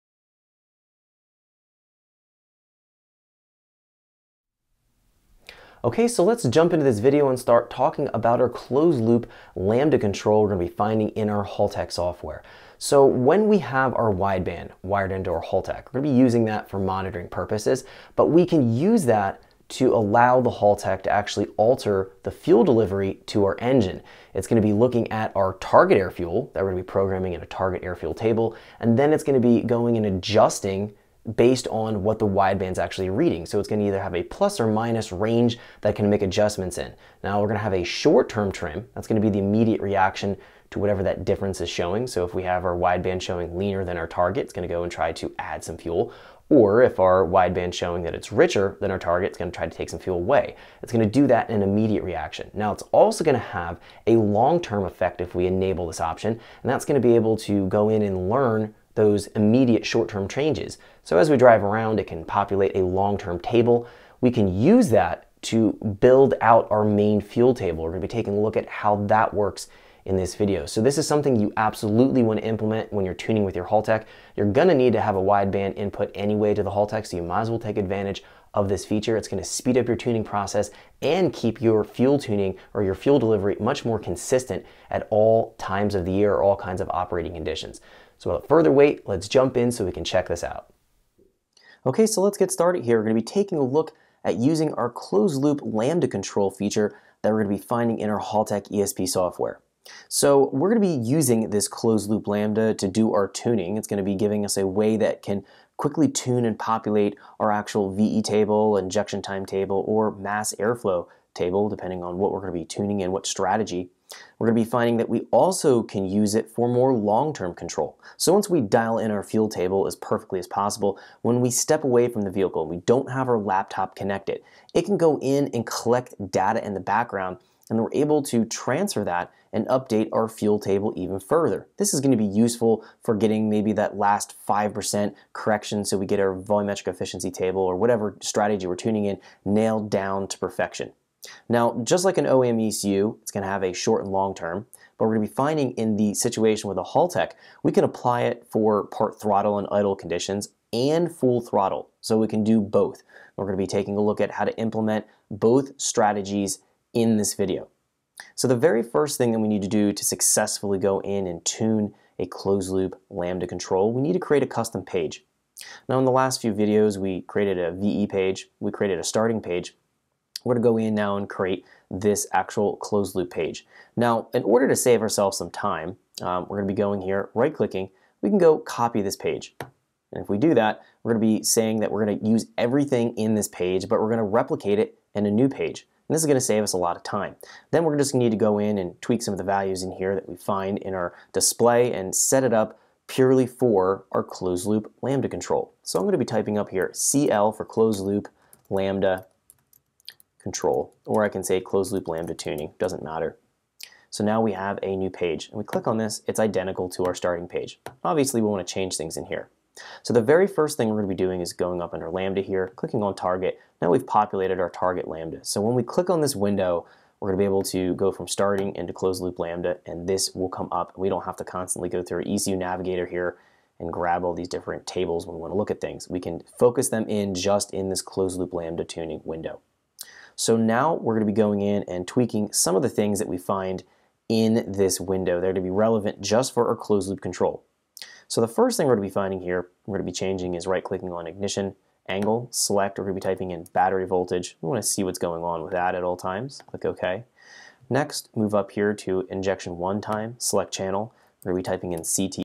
Haltech Elite: Closed Loop Lambda | Evans Performance Academy
Part 14 of the Haltech Elite training course walks you through working with the closed loop lambda control feature.   We take a look at using both the short & long term fuel trims to aid us tuning our main VE table.